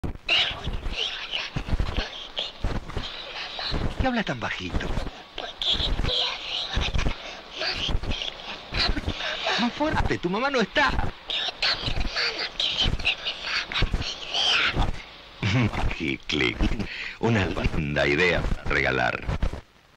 Publicidad de MAGICLICK (04/01/1984)
Gracias al proyecto personal de digitalización y recuperación de materiales históricos de los canales de televisión de Mar del Plata que lleva a cabo de manera silenciosa y paciente Fernando Delia es que llega a nuestras manos esta hermosa copia semi-podrida rescatada de la basura con el comercial de Magiclick de la empresa Aurora de 1984. En memoria de su diseñador Hugo Kogan es que le venimos a mostrar aquí.

Obsoleta :)